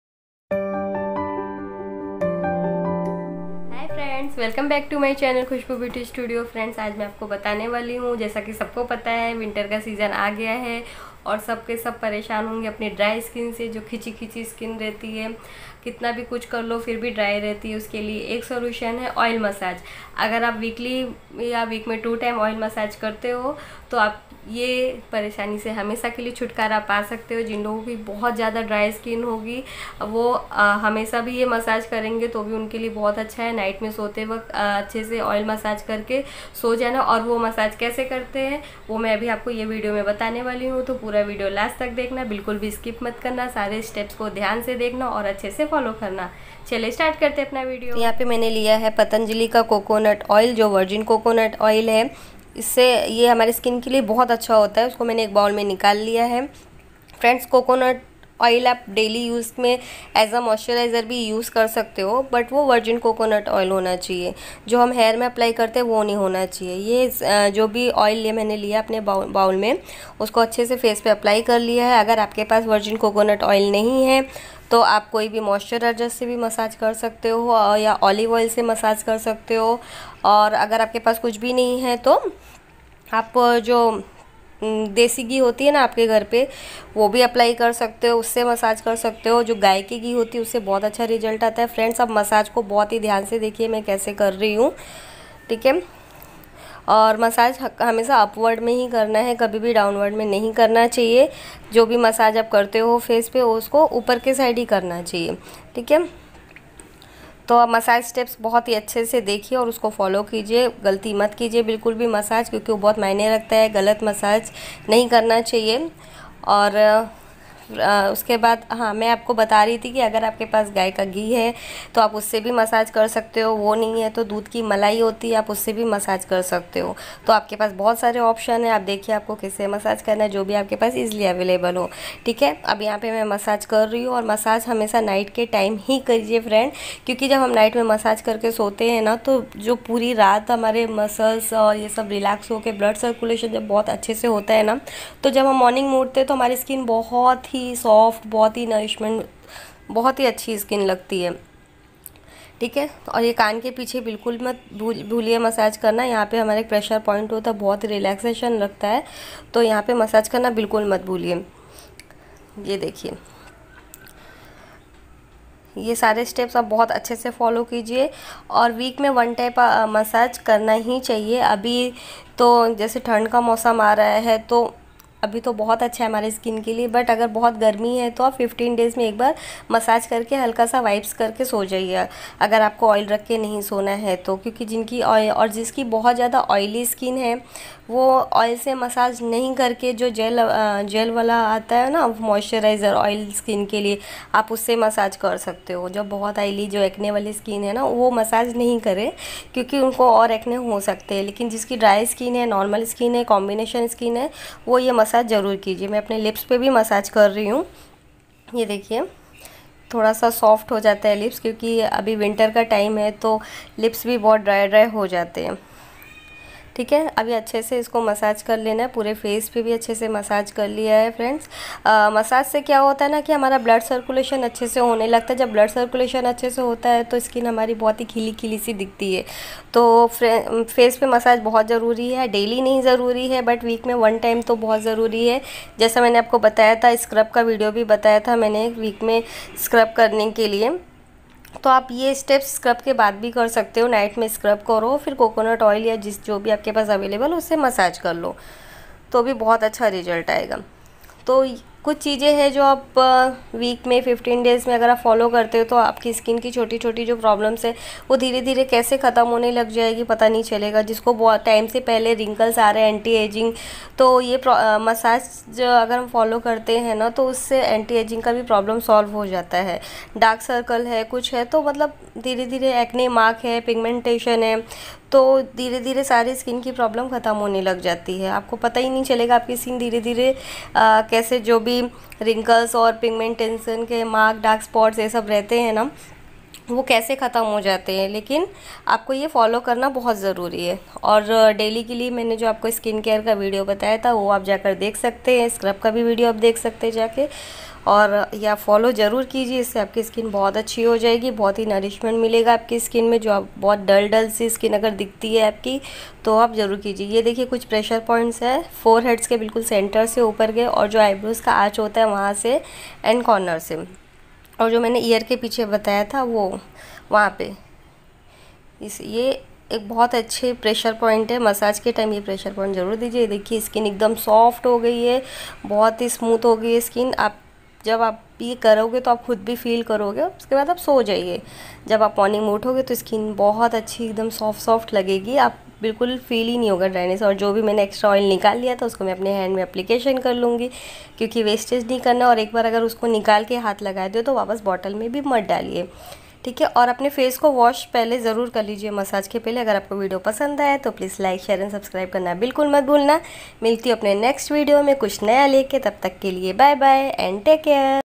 Do this coconut oil face massage for 5min.& look younger | Virgin coconut oil || Patanjali oil
Hey guys
This is very effective and useful coconut oil face massage which is very help for your face it will moisturize and ur face will look younger, and glowing soooooo try this before going to bed atleast 5min. and see result